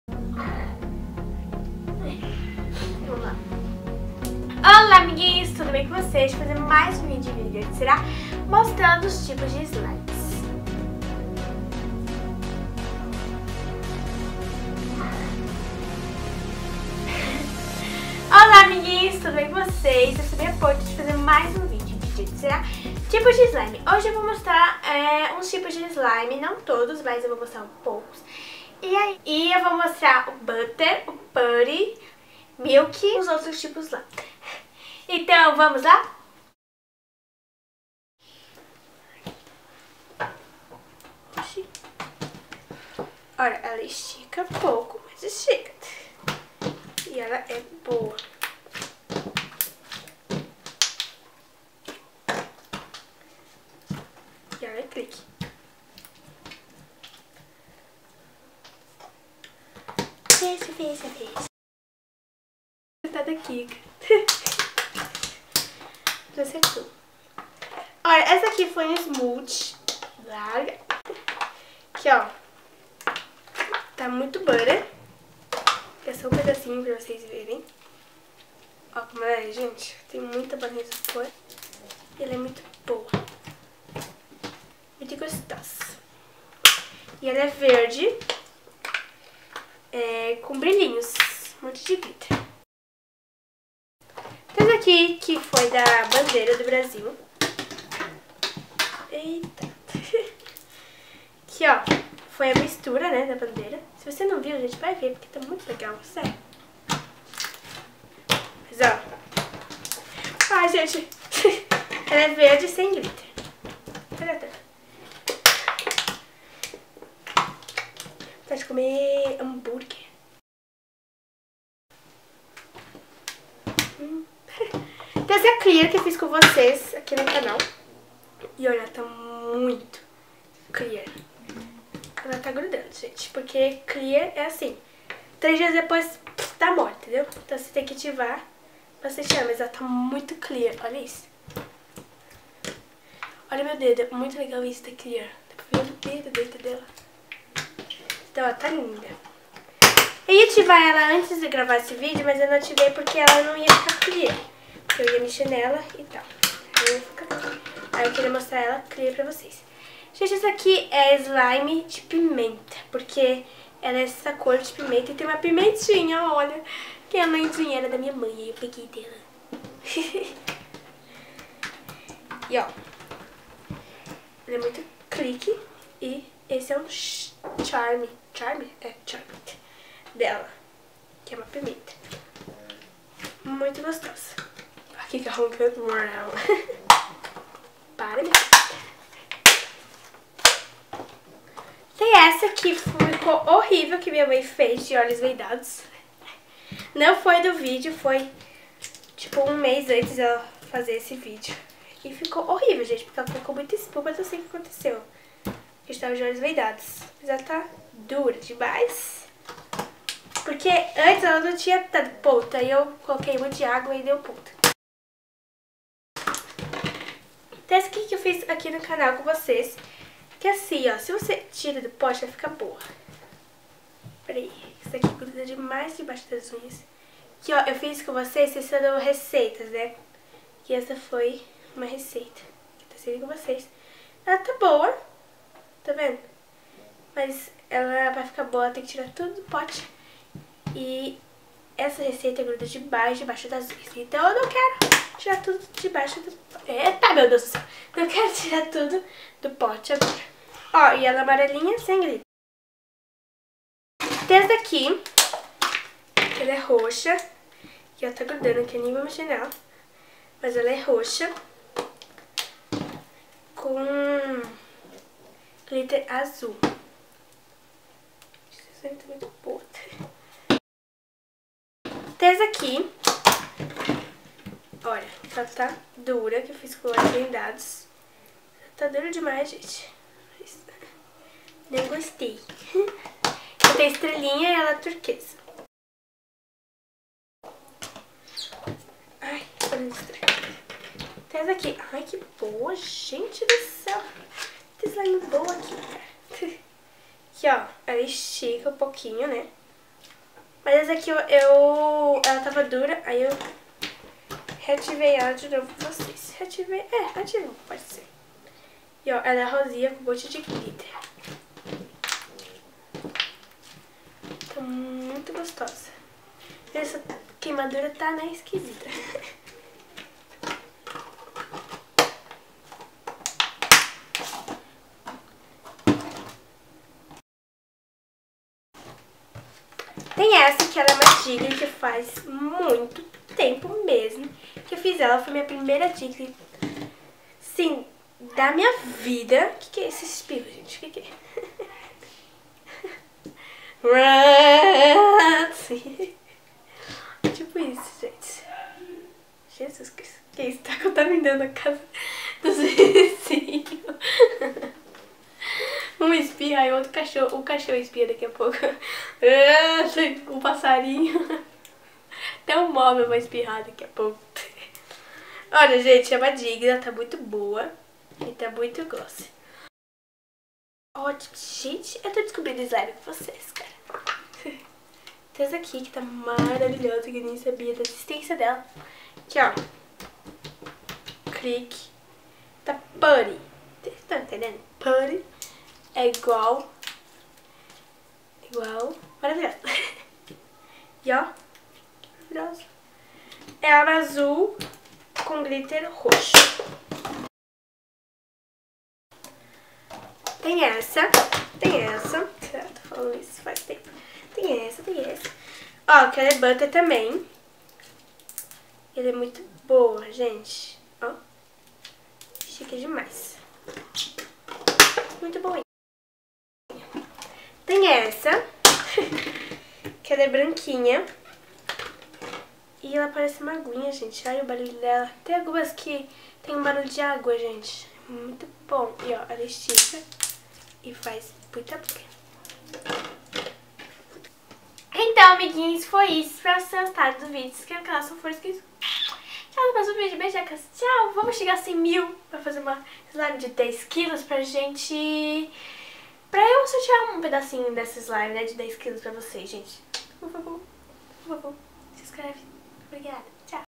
AS MELHORES SLIMES, BUTTER, MILK, CRUNCH, CLEAR, PUTTY, FLUFFY
Olá amiguinhos, nesse vídeo eu mostro os tipos de slimes que eu já fiz, são muitas slimes uma mais fofa e bonita que a outra. Espero que gostem, porque eu amei demaisssssss. Beijinhos